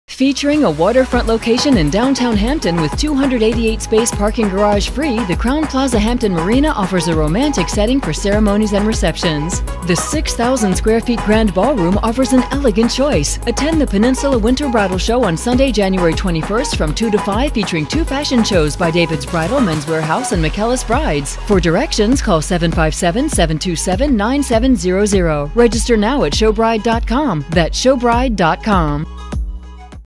Peninsula Winter Bridal Expo at Crowne Plaza Hotel Hampton Marina
Featuring a waterfront location in downtown Hampton with 288 space parking garage free, the Crowne Plaza Hampton Marina offers a romantic setting for ceremonies and locations. The 6000 sq. ft. Grand Ballroom offers an elegant choice. Attend the Peninsula Winter Bridal Expo on Sunday January 21st from 2pm to 5pm featuring David’s Bridal, Men’s Wearhouse and McEllis Brides. Register now at showbride.com